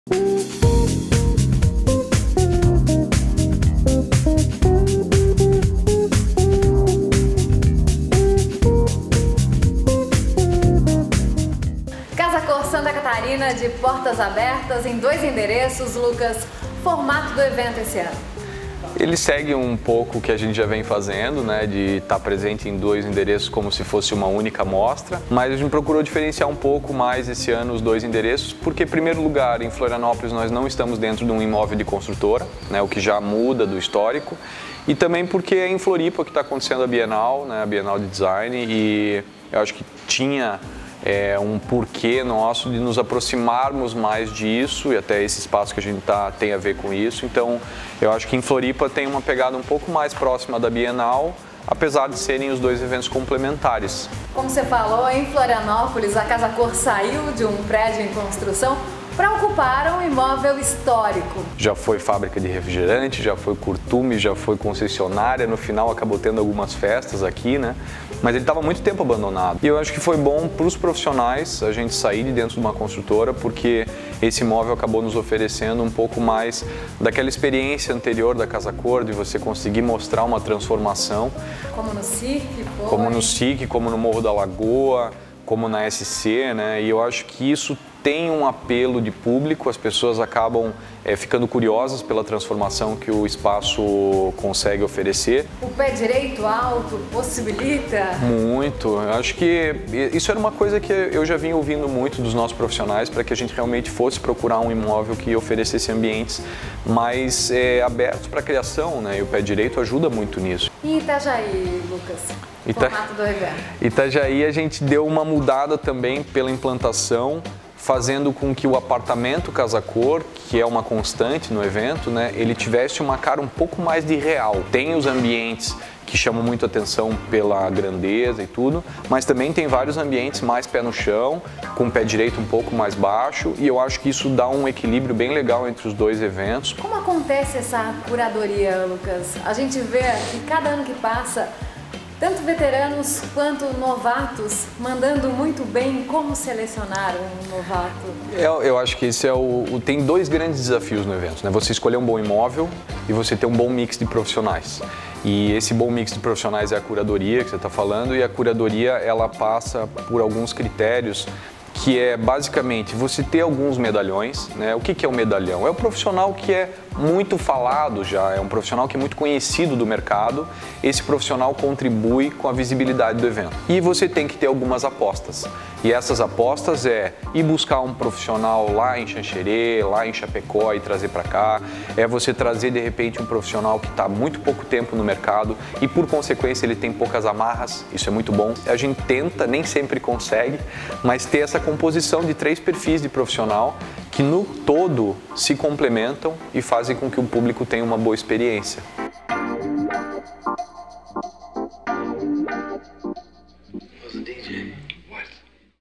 Casa Cor Santa Catarina 2015
O programa Casa e Cia começa a cobertura da mostra Casa Cor Santa Catarina, em 2015, com uma entrevosta exclusiva com Lucas Petrelli Wilmer. Conheça a proposta e os ambientes da Casa Cor em Florianópolis e Itajaí.